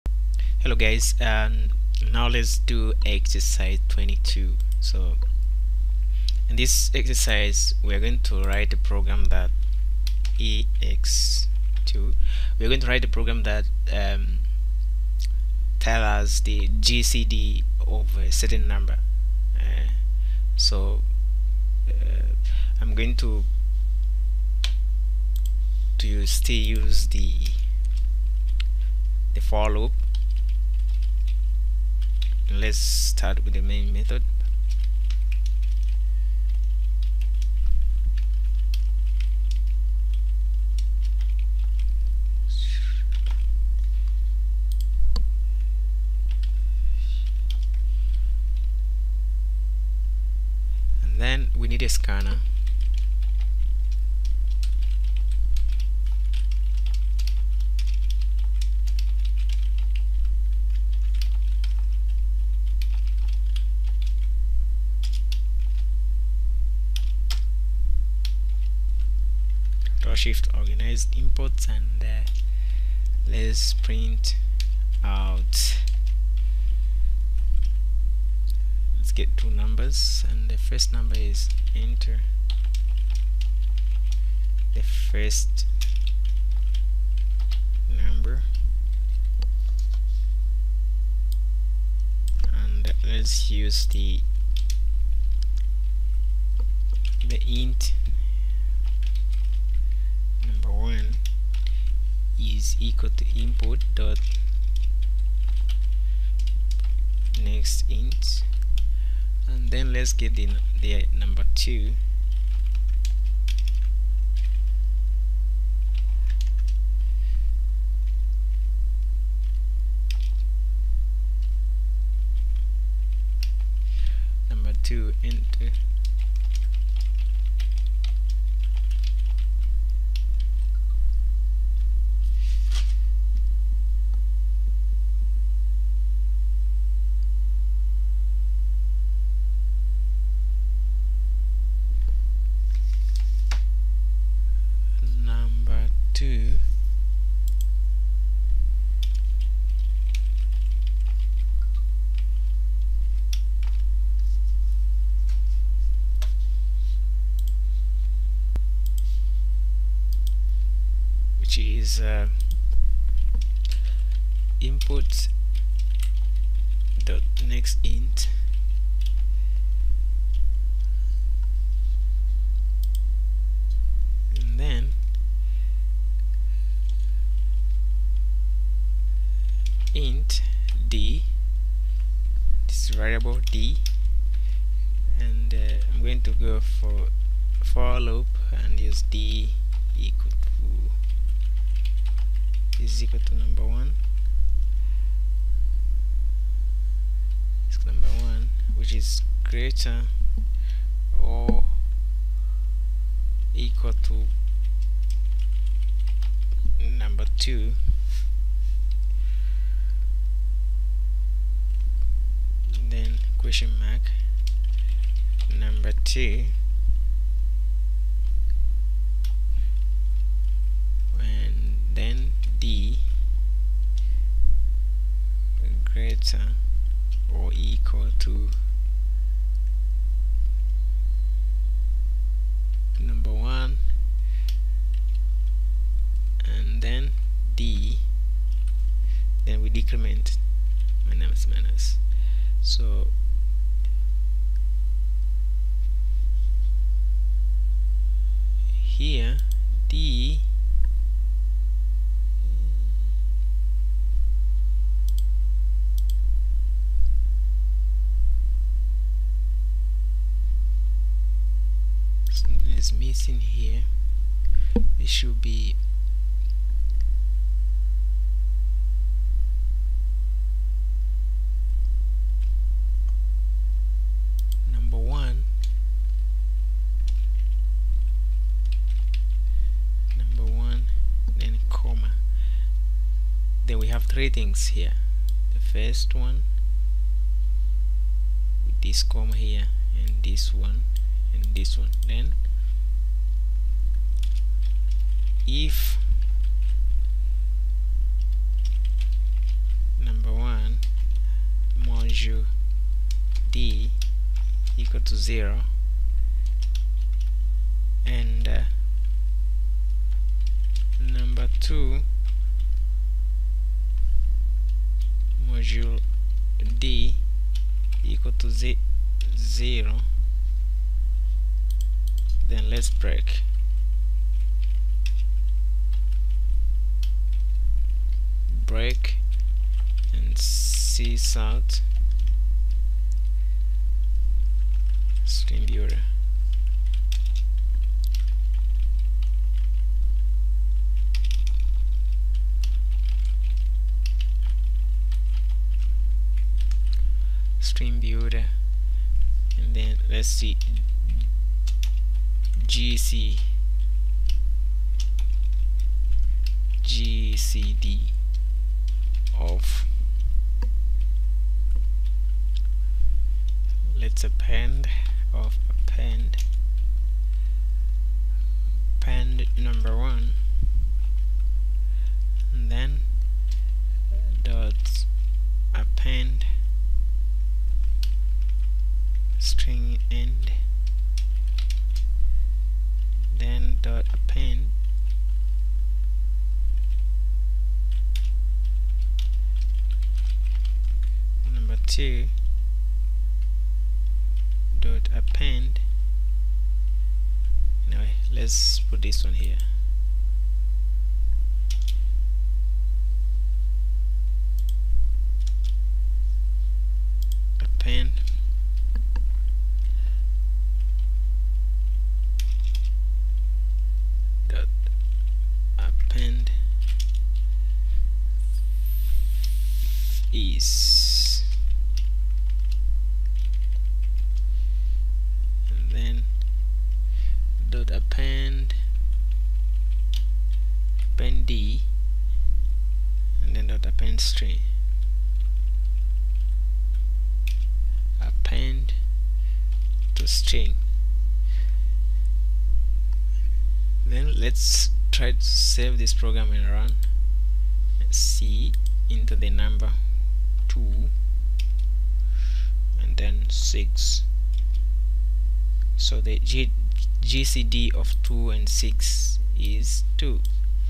Java how to look for GCD of certain numbers #22
On this exercise we are going to look for GCD of the certain numbers